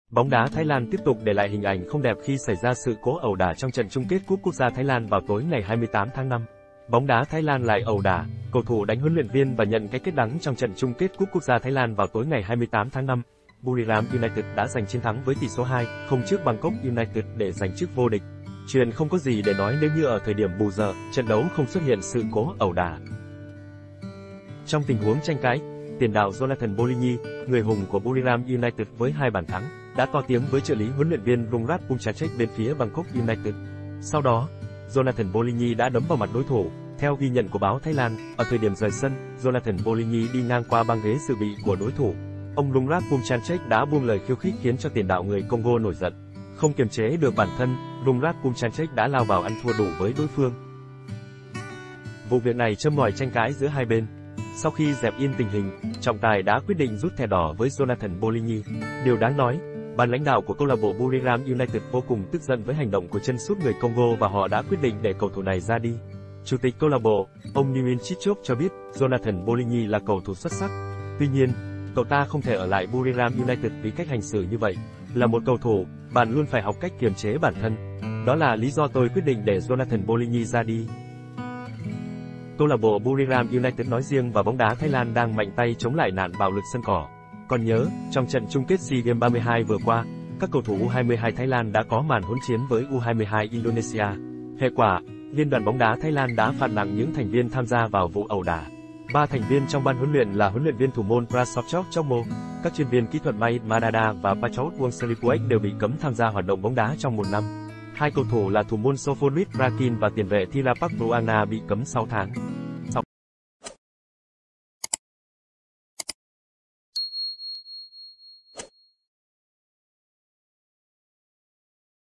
BÓNG ĐÁ THÁI LAN LẠI ẨU ĐẢ CẦU THỦ ĐÁNH HLV VÀ NHẬN CÁI KẾT ĐẮNG
Bóng đá Thái Lan tiếp tục để lại hình ảnh không đẹp khi xảy ra sự cố ẩu đả trong trận chung kết cúp quốc gia Thái Lan vào tối ngày 28 tháng 5. Bóng đá Thái Lan lại ẩu đả: Cầu thủ đánh huấn luyện viên và nhận cái kết đắng Trong trận chung kết cúp quốc gia Thái Lan vào tối ngày 28 tháng 5, Buriram United đã giành chiến thắng với tỷ số 2-0 trước Bangkok United để giành chức vô địch. Chuyện không có gì để nói nếu như ở thời điểm bù giờ, trận đấu không xuất hiện sự cố ẩu đả.